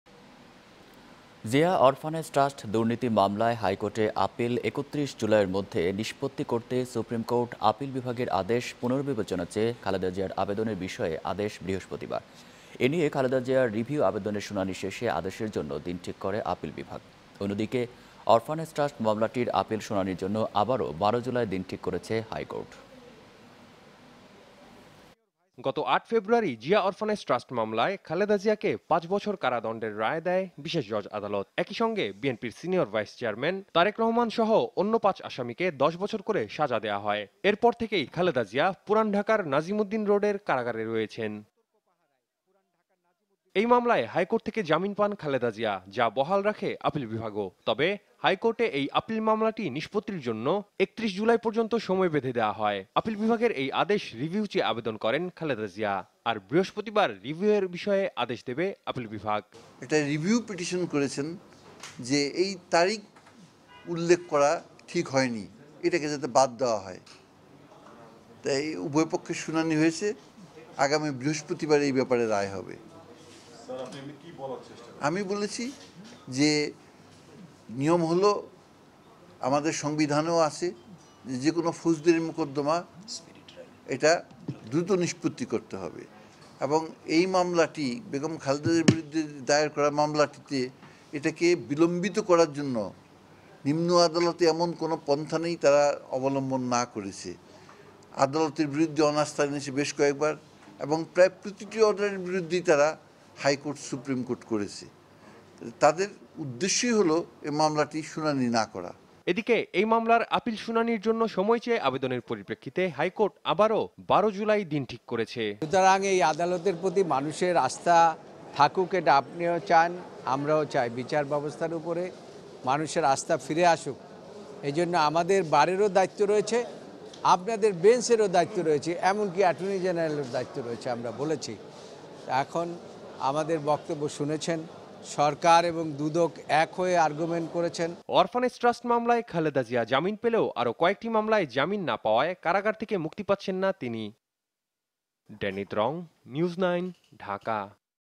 জিয়া অরফানেজ ট্রাস্ট মামলা নিস্পত্তিতে আপিলের শুনানি বৃহস্পতিবার
(নিউজ নাইন। প্রচার : সোমবার, ০৯.০৭.২০১৮)